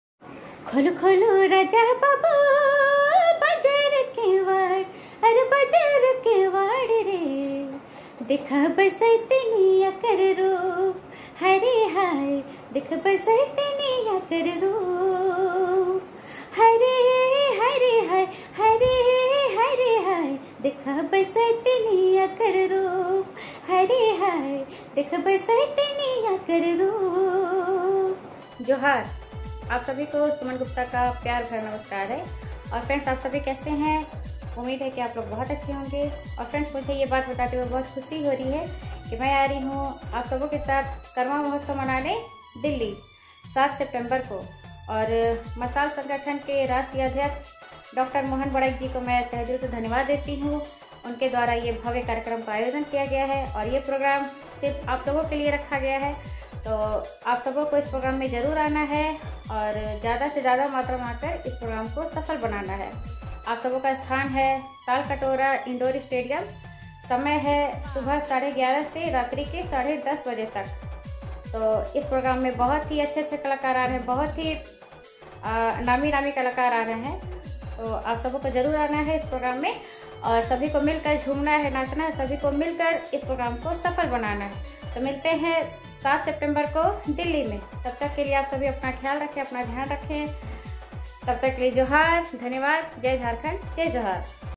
SUMANGUPTA nagpurigeet mswskarammahotsav jhollywood tranding music viralvideo
सुप्रसिद्ध आधुनिक नागपुरी लोक गायिका सुमन गुप्ता की प्रस्तुति मशाल संगठन द्वारा आयोजित तालकटोरा इंडोर स्टेडियम में करम पूर्व संध्या पूजा महोत्सव आमंत्रण वीडियो ।